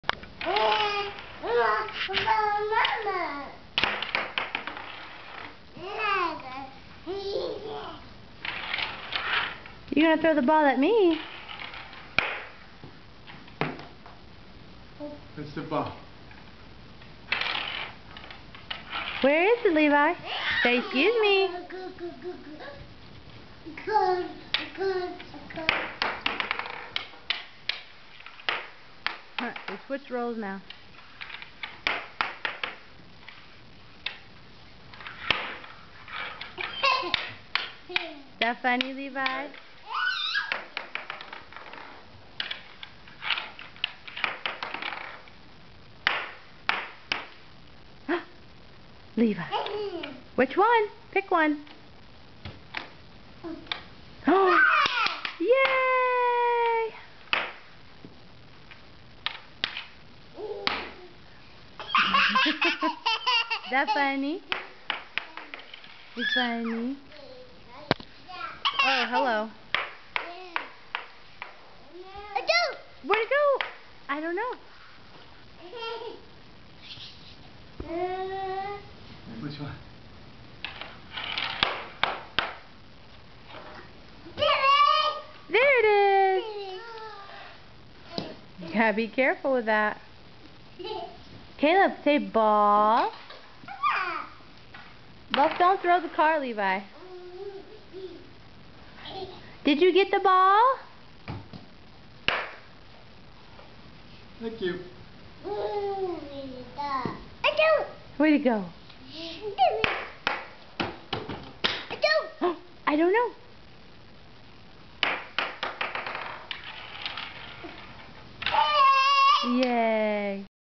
nick's visit
nick playing with the boys